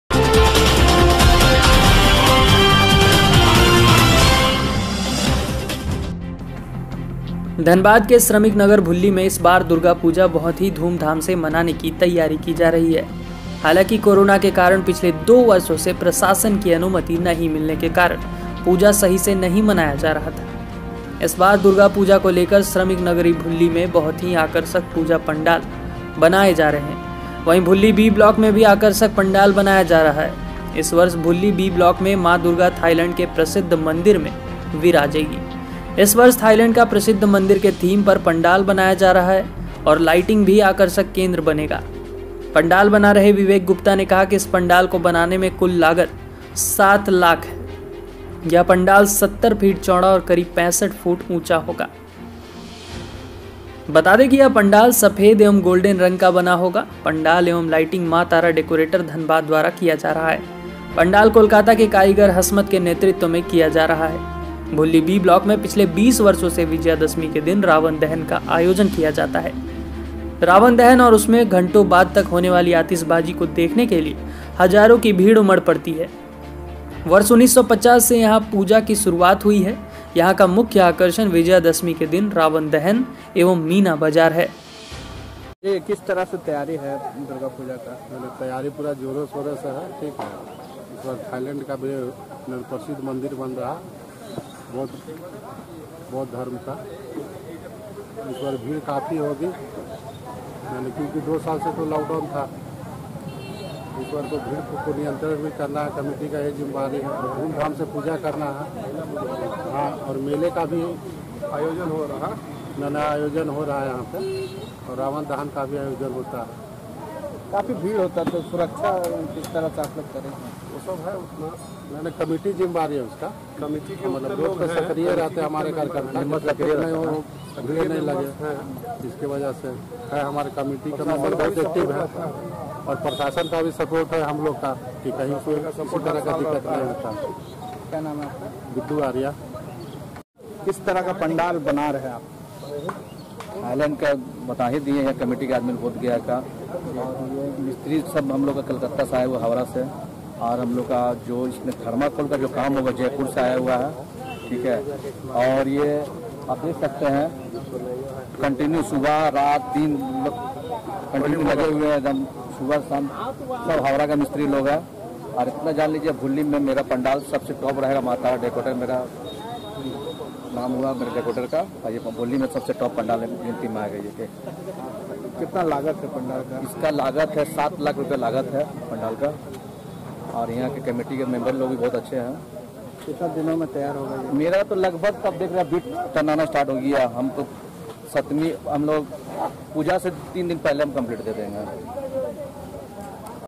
इस बार भूली में भब्य दुर्गा पूजा की तैयारी:इस बार माँ दुर्गा थाईलैंड के प्रशिद्ध मंदिर में विराजेगी
कोरोना के बाद इस बार भूली में भब्य दुर्गा की तैयारी:इस बार माँ दुर्गा थाईलैंड के प्रशिद्ध मंदिर में विराजेगी

DHANBAD:धनबाद के श्रमिक नगरी  भूली में इस बार दुर्गा पूजा बहुत ही धूम धाम से मानाने की तैयारी की जा रही है | हालांकि कोरोना के कारण पिछले 2 वर्षों से प्रशासन की अनुमति नहीं मिलने के कारण  पूजा   सही से नहीं मनाया जा रहा था |

इस बार दुर्गा पूजा को लेकर  श्रमिक नगरी भूली में  बहुत ही आकर्षक पूजा पंडाल बनाए जा रहे हैं | वही भूली बी ब्लॉक में भी  आकर्षक पंडाल बनाया जा रहा है  ।

इस वर्ष भूली बी ब्लॉक में माँ दुर्गा थाईलैंड के प्रशिद्ध मंदिर में विराजेगी | इस वर्ष थाईलैंड का प्रसिद्ध मंदिर के थीम पर पंडाल बनाया जा रहा है  और लाइटिंग  भी आकर्षक  केंद्र बनेगा पंडाल बना रहे मां तारा डेकोरेटर के  विवेक गुप्ता ने कहा कि  इस पंडाल को बनाने में कुल लागत ₹700000 है ।यह पंडाल  70 फीट चौड़ा और करीब 65 फुट ऊंचा होगा | यह पंडाल सफेद एवं गोल्डन रंग का बना होगा,पंडाल एवं लाइटिंग मां तारा डेकोरेटर धनबाद द्वारा किया जा रहा है | पंडाल कोलकाता के कारीगर हसमत के नेतृत्व में काम किया जा रहा है

भूली बी ब्लॉक में पिछले 20 वर्षों से विजयादशमी के दिन रावण दहन का आयोजन किया जाता है, रावण दहन और उसमें घंटों बाद तक होने वाली आतिशबाजी को देखने के लिए हजारों की भीड़ उमड़ पड़ती है| वर्ष 1950 से यहां पूजा की शुरुआत हुई यहां का मुख्य आकर्षण विजयादशमी के दिन रावण दहन एवं मीना बाजार है| हालांकि कोरोना के कारण पिछले 2 वर्षों से प्रशासन की अनुमति नहीं मिलने के कारण रावण दहन नहीं हुआ था |  यहां मुख्य आकर्षण पंडाल के बगल स्थित एमपी आई ग्राउंड में लगने वाला मीणा बाजार होता है, कमेटी में अध्यक्ष रोशन कुमार उर्फ मिंटू सचिव गुड्डू आर एवं कोषाध्यक्ष नीरज शर्मा है।